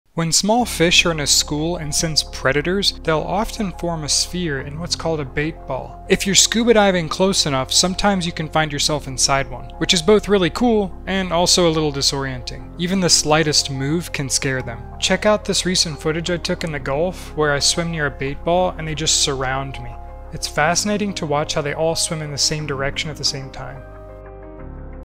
SCUBA diving inside a school of fish called a Bait Ball
In this video I explain what a bait ball is and what it looks like from the outside and inside.